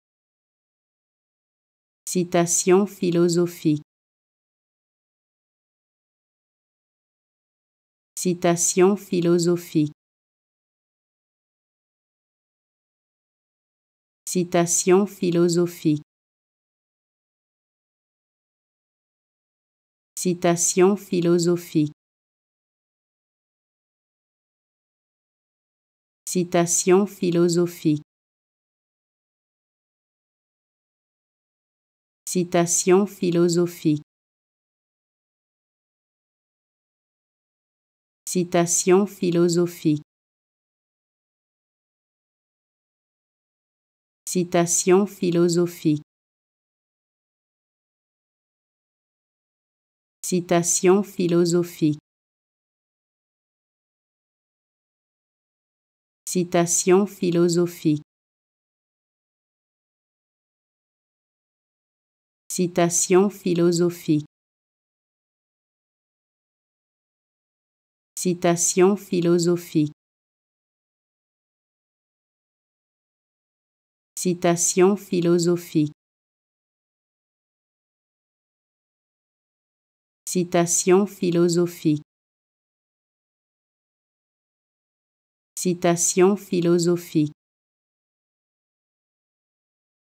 How to Pronounce ''Citation philosophique'' (Philosophical quote) Correctly in French
Learn how to say and properly pronounce ''Citation philosophique'' in French with this free pronunciation tutorial.